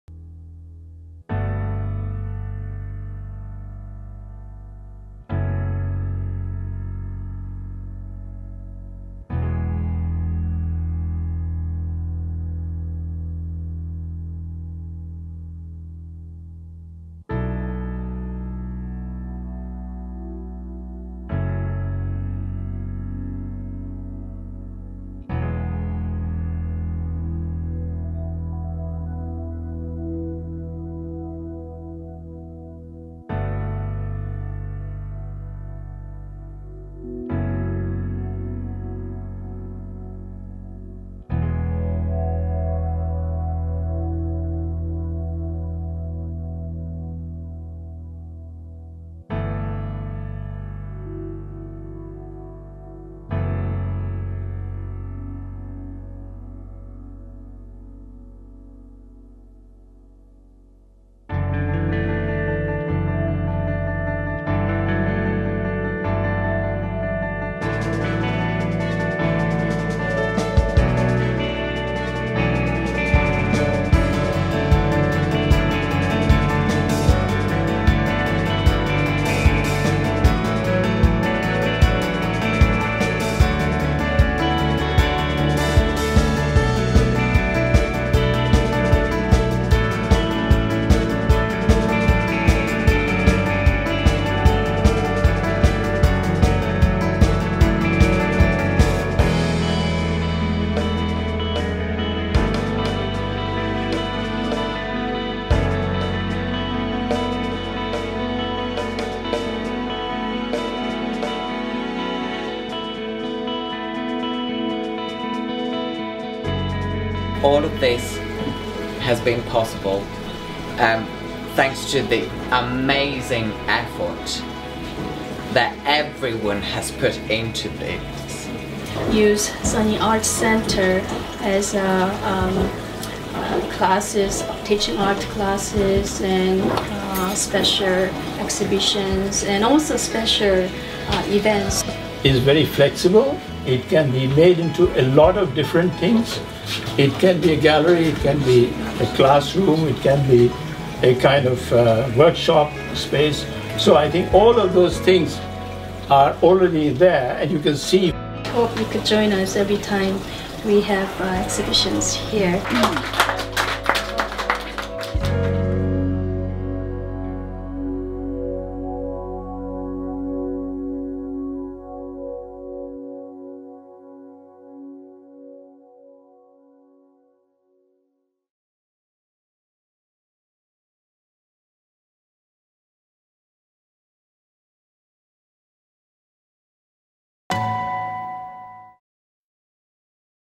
Sunny Art Centre Opening Exhibition: OVERTURES
Sunny Art Centre is pleased to announce Overtures: Representing Art from Near and Far, a group exhibition which will inaugurate our new space in Chancery Lane, London. The exhibition will include works by international artists showcasing  an “orchestra” of skills, techniques and mediums. Our opening show, in which painting will meet the art of fine ceramic and jewellery, will embody ideals of beauty, harmony, and diversity in art. SAC is proud to be hosting this effort in bridging different artistic communities from around the world.